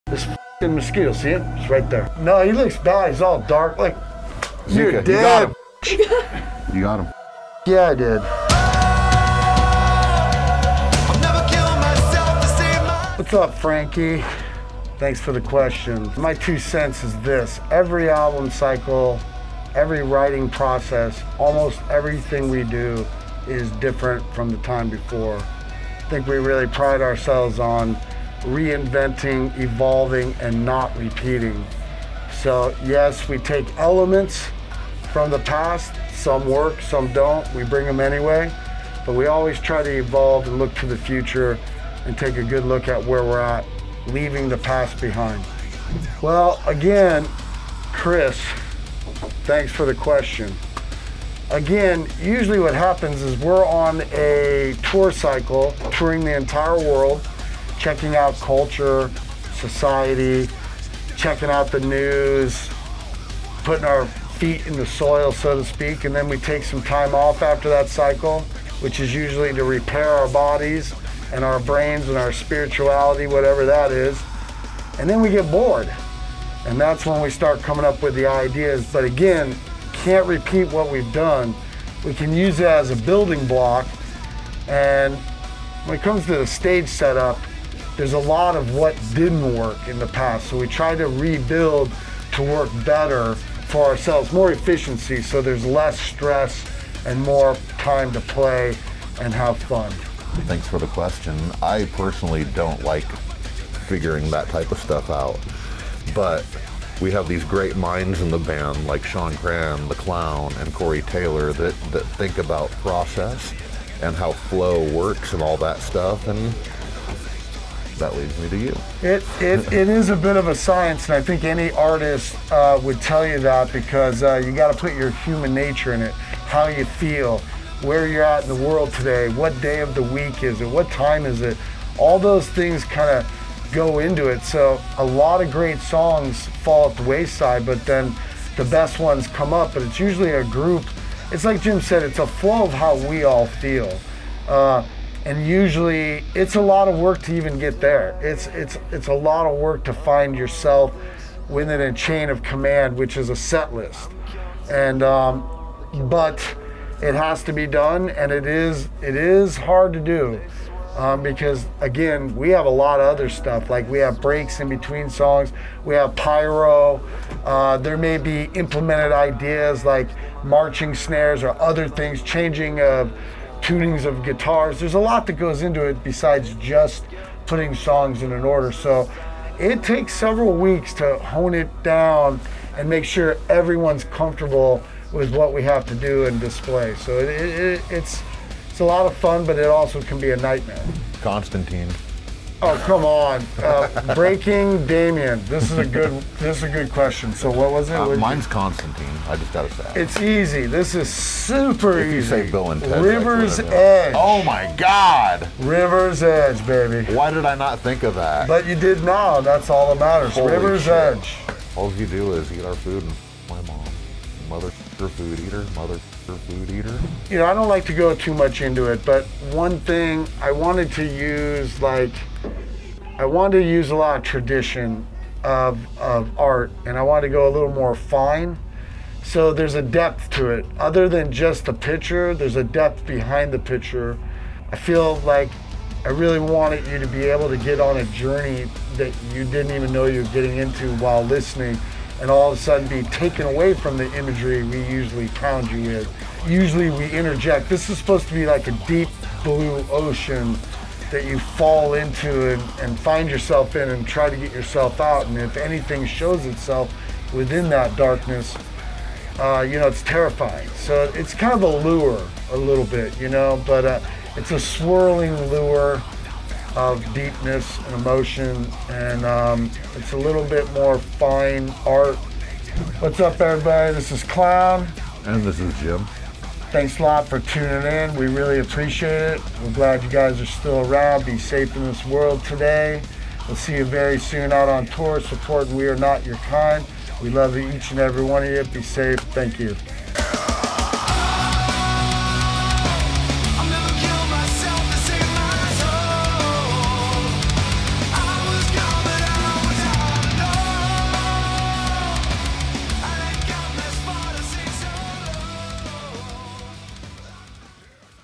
5Q4: Slipknot
We sat down with Jim and Clown of Slipknot to chat about their writing process, unique details about their stage setup, and which Keanu Reeves movie is their favourite. Watch 5Q4Slipknot now.